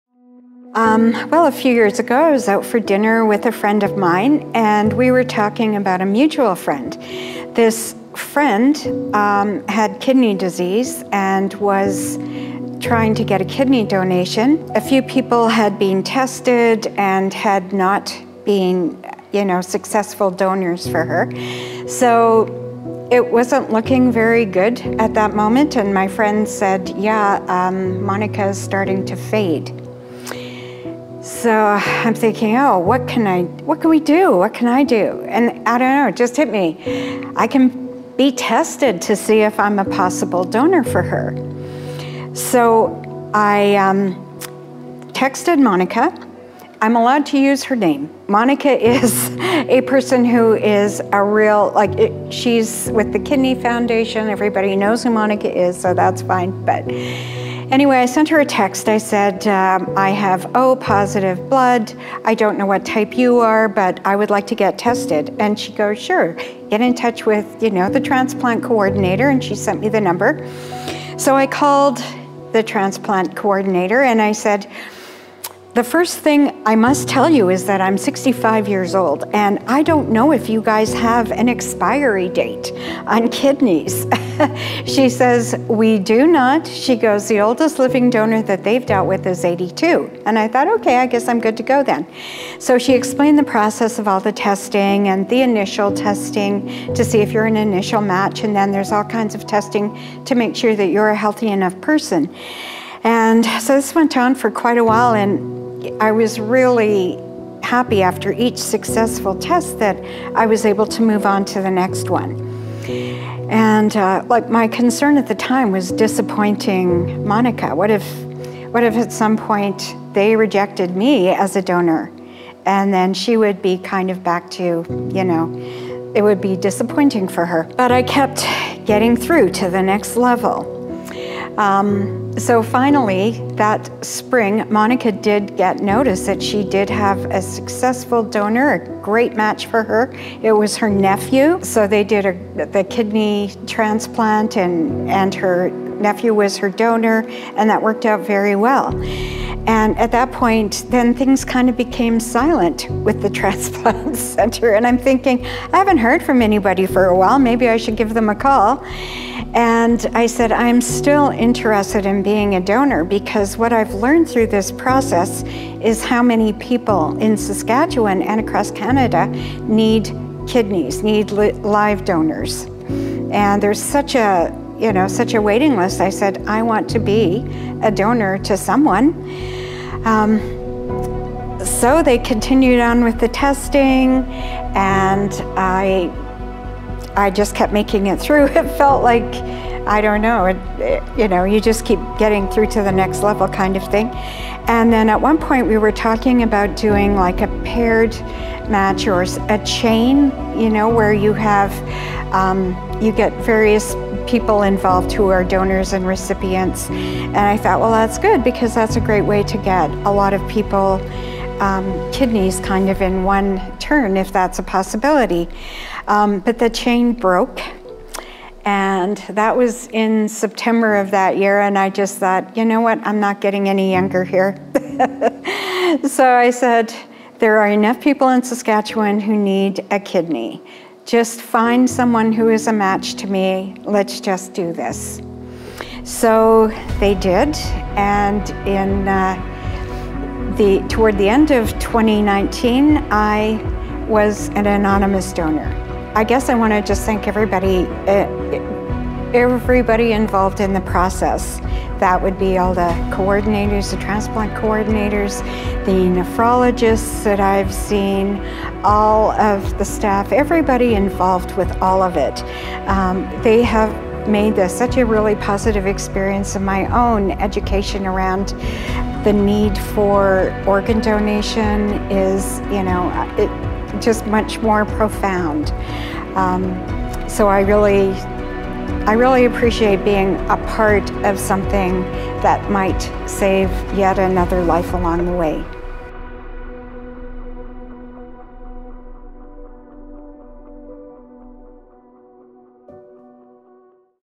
Meet Marie, a kidney donor who gave the gift of life to a complete stranger.
Meet Marie, a kidney donor who chose to undergo vigorous testing and workups to give the gift of a life saving donation to a complete stranger.

To learn more about living organ donation, visit us at greatactions.ca